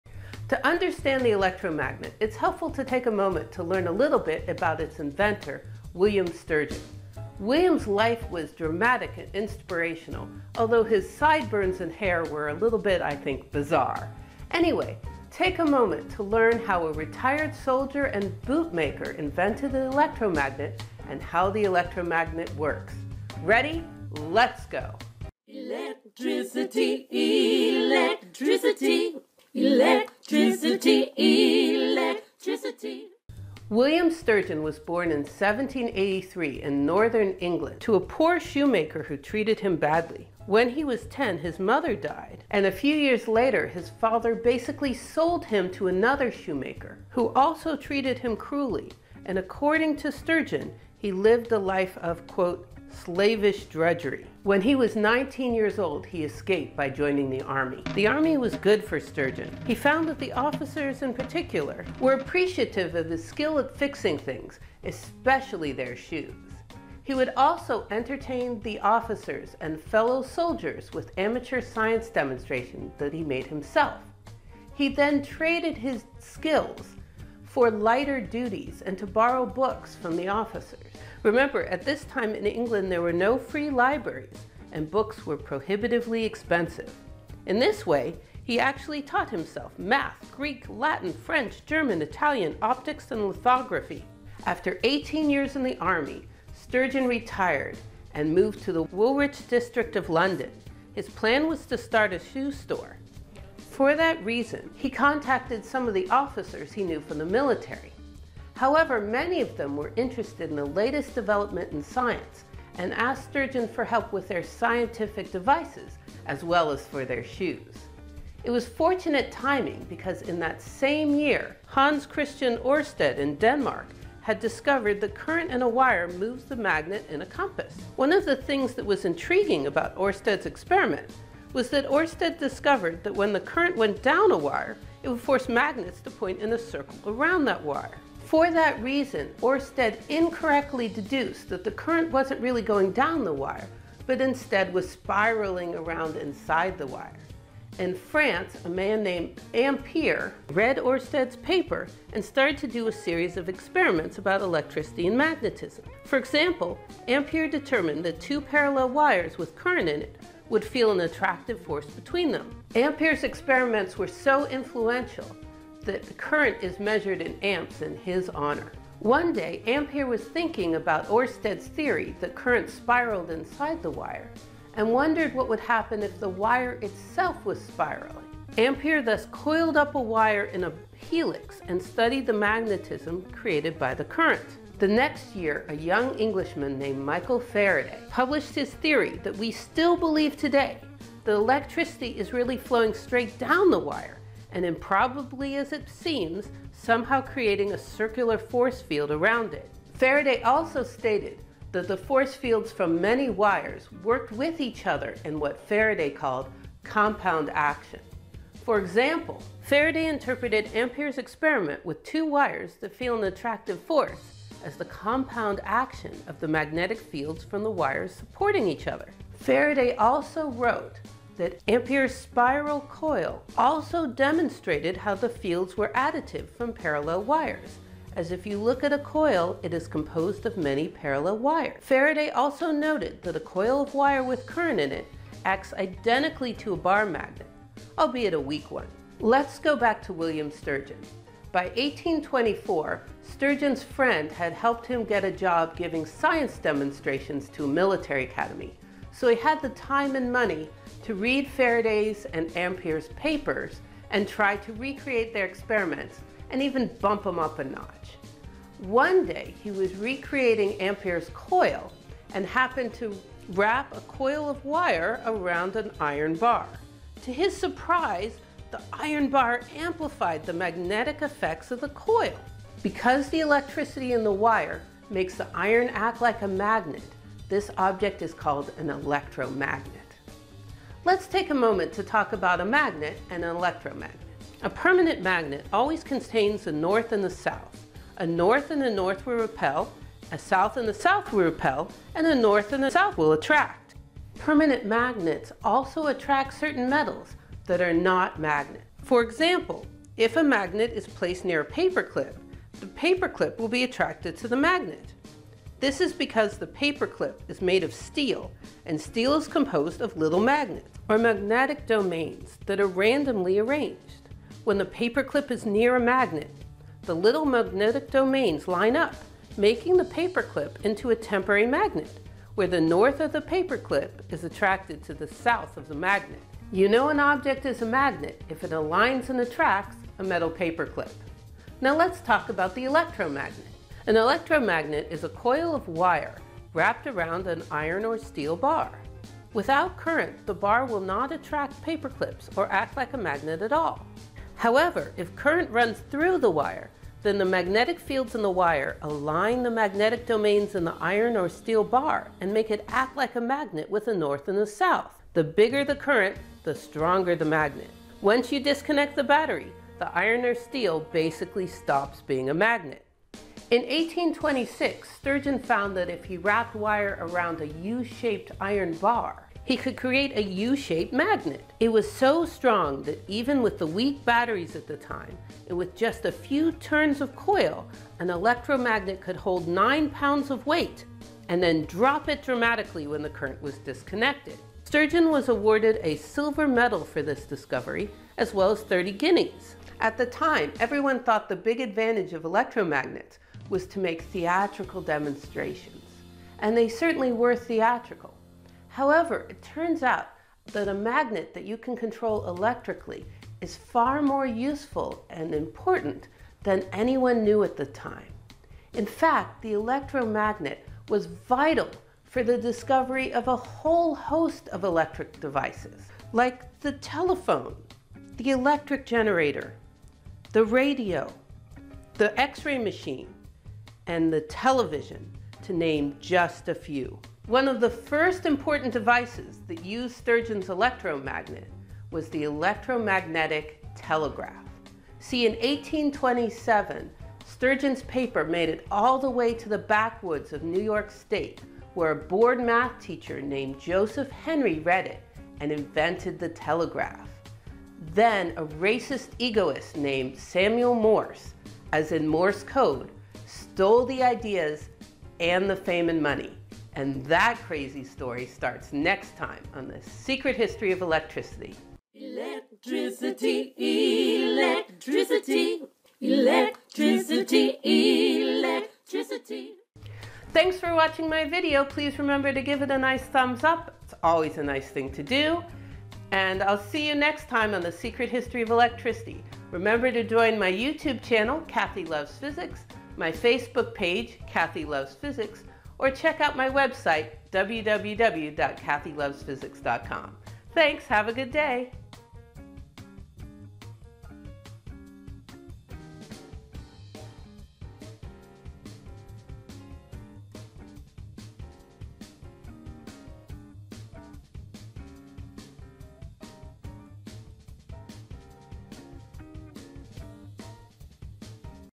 How an Electromagnet Works and Why a Shoemaker Invented it
Why did a shoemaker and retired soldier named William Sturgeon invent the electromagnet and how does an electromagnet work anyway?  Watch this video to find out!

As usual thanks to Kim Nalley for the fabulous singing.